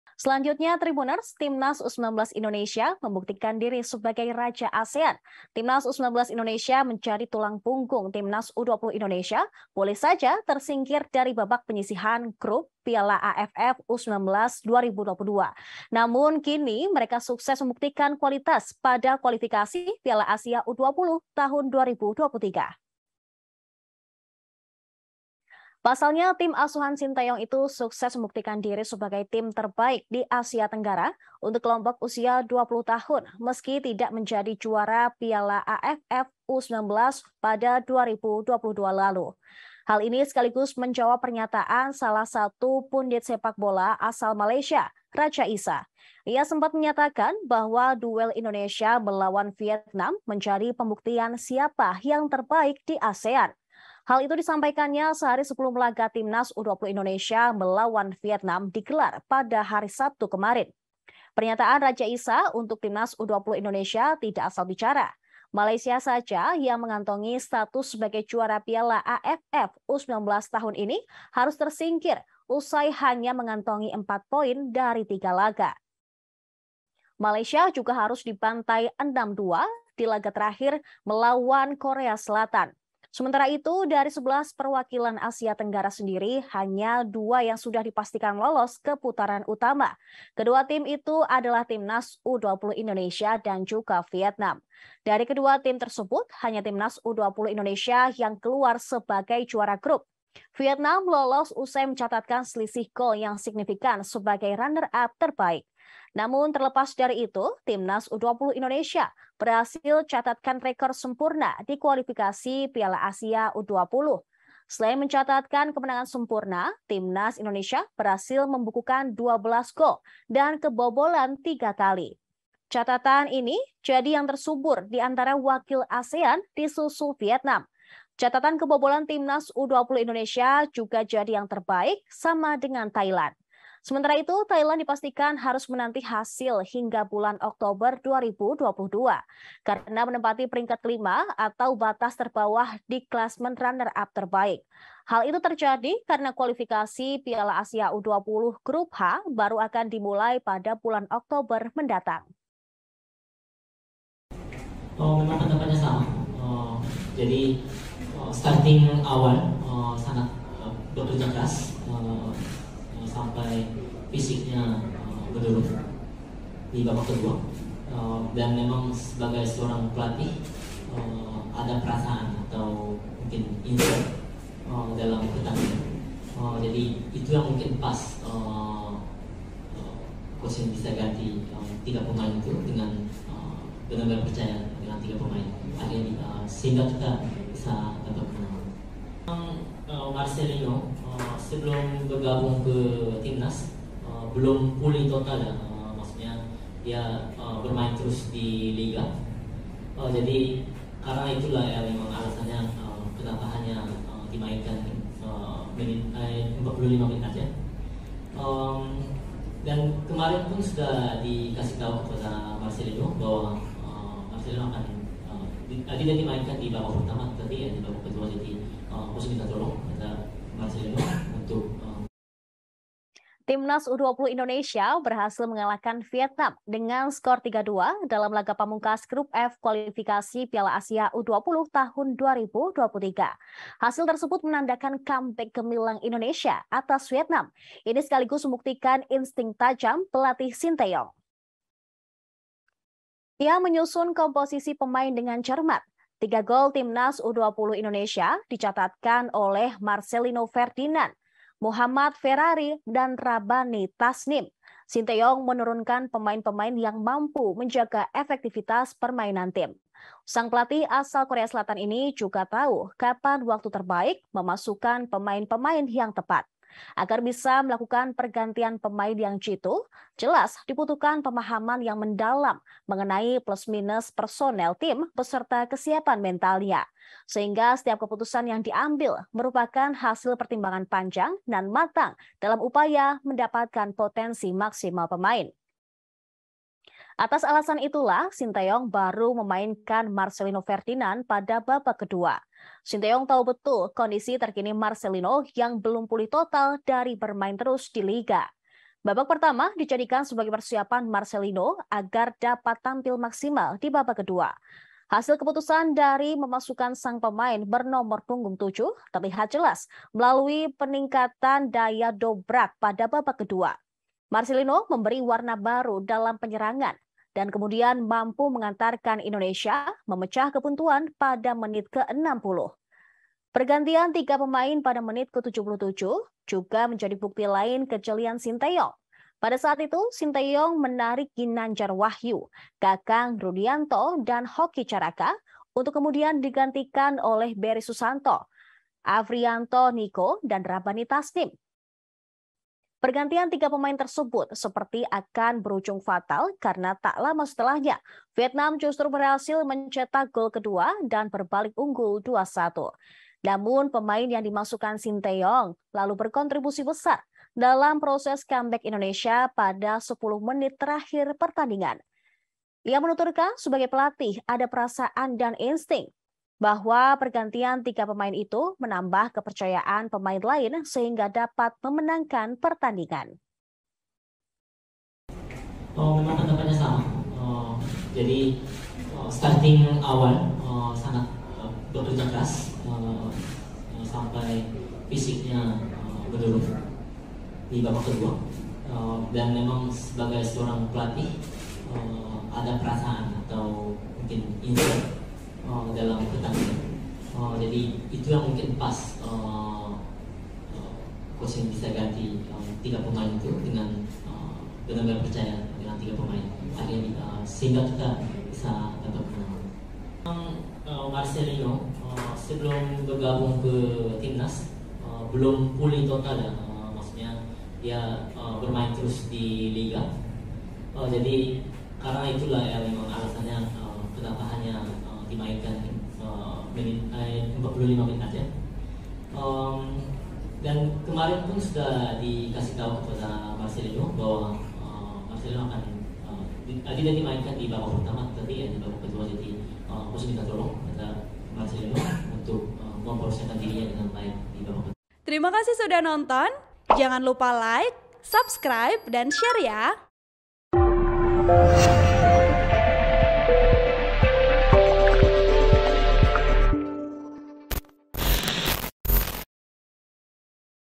Kalahkan Vietnam 3 2 di Kualifikasi Piala Asia U20, Timnas U19 Indonesia Buktikan Jadi Raja ASEAN
TRIBUN-VIDEO.COM - Timnas U19 Indonesia membuktikan diri sebagai raja ASEAN.

Timnas U-19 Indonesia yang jadi tulang punggung timnas U-20 Indonesia boleh saja tersingkir dari babak penyisihan grup Piala AFF U-19 2022.

Namun, timnas U-20 Indonesia sukses buktikan kualitas mereka pada Kualifikasi Piala Asia U-20 2023.

Pasalnya, tim asuhan Shin Tae-yong itu sukses buktikan diri sebagai tim terbaik di Asia Tenggara untuk kelompok usia 20 tahun meski tidak jadi juara Piala AFF U-19 2022.

Hal ini sekaligus menjawab pernyataan salah satu pundit sepak bola asal Malaysia, Raja Isa.

Ia sempat menyatakan bahwa duel Indonesia melawan Vietnam jadi pembuktian siapa yang terbaik di ASEAN.

Hal itu disampaikannya sehari sebelum laga timnas U-20 Indonesia lawan Vietnam digelar pada Sabtu (17/9/2022).

Pernyataan Raja Isa untuk timnas U-20 Indonesia tidak asal bicara.

Malaysia saja yang mengantongi status sebagai juara Piala AFF U-19 tahun ini saja harus tersingkir usai hanya kantongi empat poin dari tiga laga.

Malaysia juga harus dibantai 6-2 di laga terakhir lawan terberat, Korea Selatan.

Sementara itu, dari 11 perwakilan Asia Tenggara sendiri, hanya dua yang sudah dipastikan lolos ke putaran utama.

Kedua tim tersebut adalah timnas U-20 Indonesia dan Vietnam.

Dari kedua tim tersebut, hanya timnas U-20 Indonesia yang keluar sebagai juara grup.

Vietnam lolos usai mencatatkan selisih gol yang signifikan sebagai runner-up terbaik.

Namun terlepas dari itu, timnas U-20 Indonesia berhasil catatkan rekor sempurna di Kualifikasi Piala Asia U-20.

Selain mencatatkan kemenangan sempurna, timnas U-20 Indonesia berhasil membukukan 12 gol dan kebobolan 3 kali.

Catatan gol timnas U-20 Indonesia jadi yang tersubur diantara wakil ASEAN disusul Vietnam.

Catatan kebobolan timnas U-20 Indonesia juga jadi yang terbaik, sama dengan Thailand.

Sementara Thailand, dipastikan harus menanti hasil hingga bulan Oktober 2022.

Karena menempati peringkat kelima atau batas terbawah di klasemen runner-up terbaik.

Hal itu terjadi, karena Kualifikasi Piala Asia U-20 Grup H baru dimulai pada bulan Oktober 2022.(*)

Host : Saradita
Vp : Dedhi Ajib Ramadhani